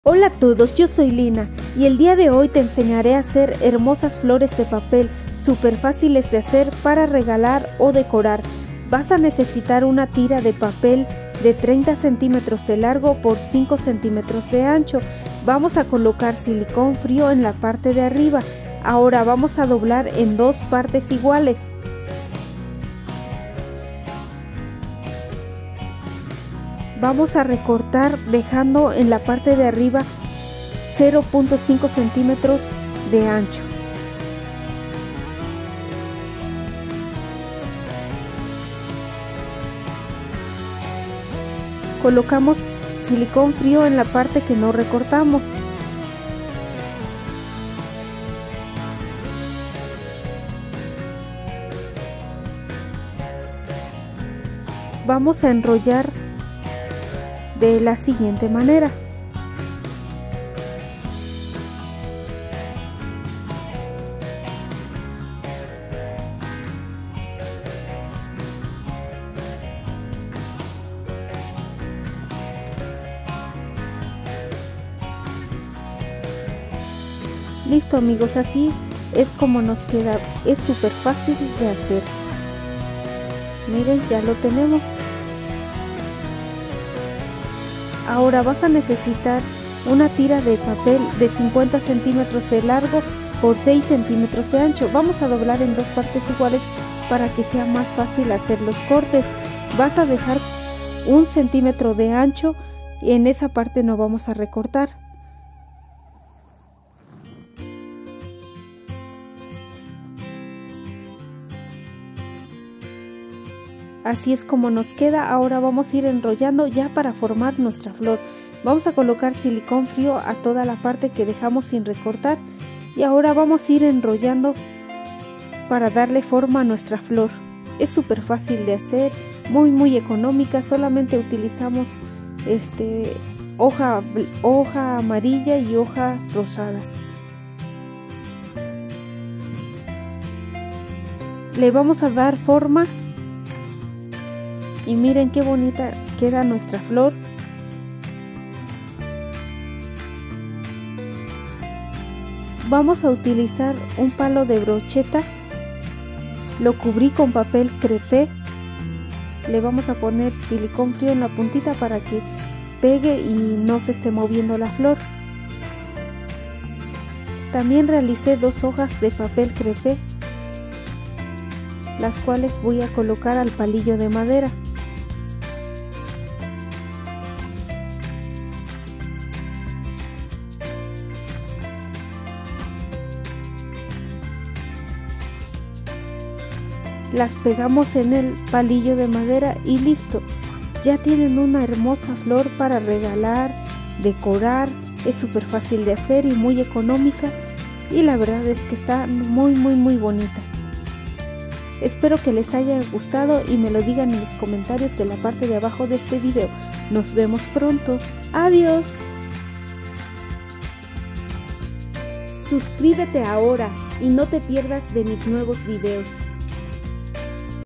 Manualidades: FLORES de PAPEL Fácil de Hacer (Margaritas) - Manualidades de Lina
Manualidades con papel - Aprende hacer estas hermosas flores de papel
fáciles (Margaritas) para decorar tu hogar o regalar en el día de la madres o en cualquier fecha especial. Espero les guste!!

♥♥♥ MAS MANUALIDADES ♥♥♥

Créditos:
Año: 2023
Prohibida la reproducción total o parcial de este vídeo